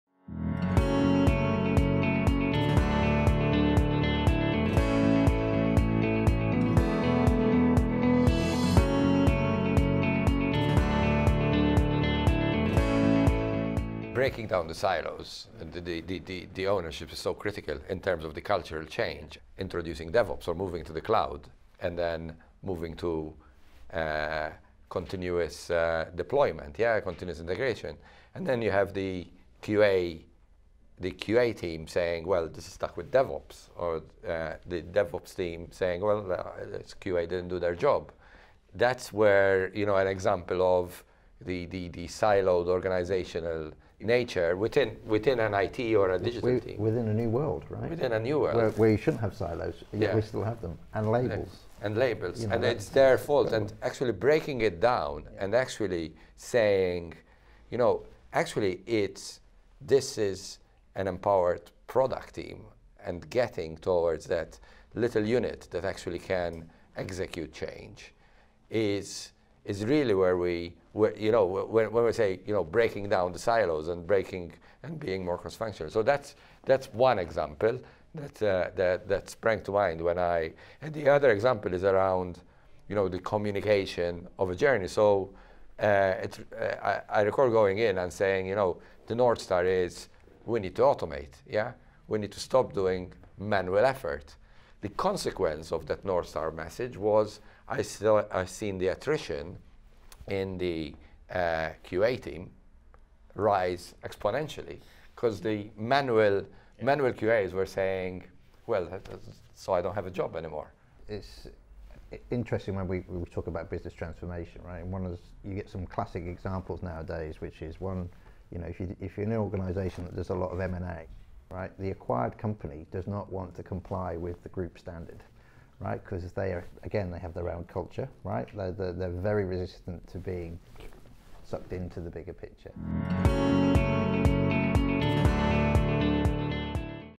Why collaboration is key to any cultural transformation
If a company is going to evolve its culture successfully, it needs to improve its collaborative skills. Mark Brincat, CTO, SHL and Dave Cook, CDO, Croda, at the time of filming, discuss. Filmed at The Studio, partnered with Rackspace.